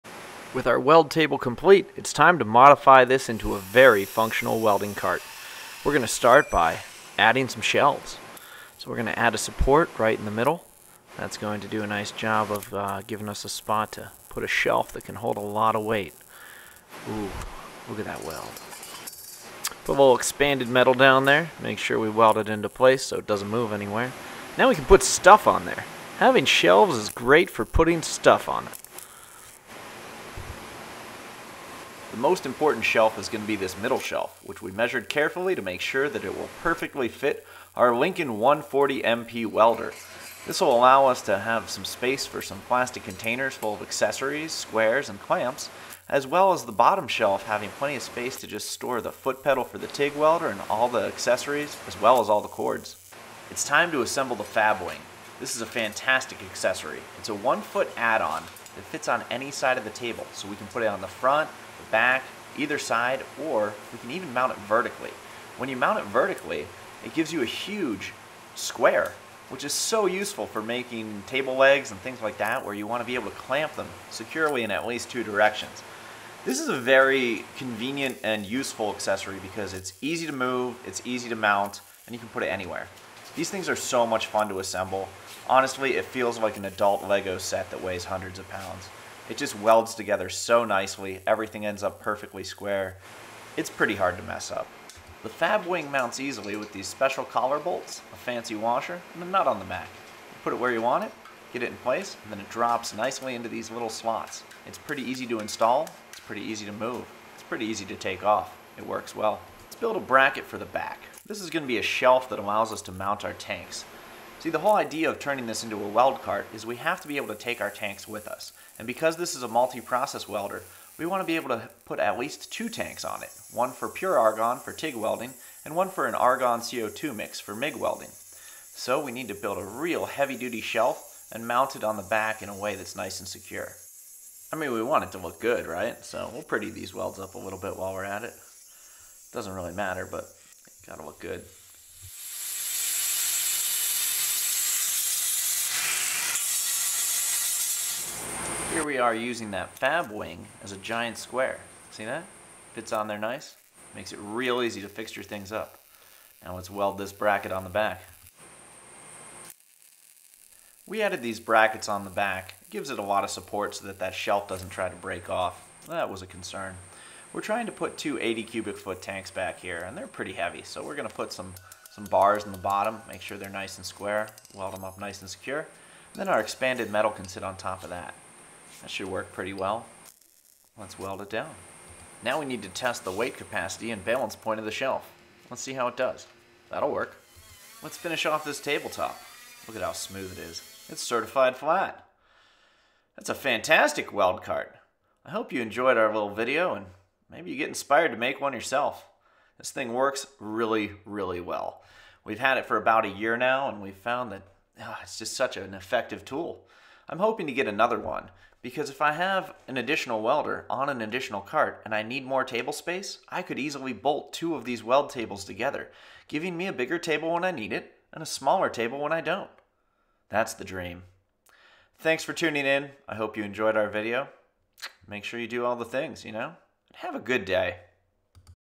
Weld Table Part 2- The Ultimate DIY Weld Cart
Part two of our Ultimate Weld Cart! Built out of a 24''x24'' "Fab Block" from Certiflat weld tables, this became the most useful weld cart available to our small shop space. This was a blast to build!

These kits are very easy to build and a ton of fun to put together. Everything snaps into place easily and stays nice and square. It's pretty hard to imagine screwing this up- you've got this!
If you need a weld table, or a great welding cart, look no further!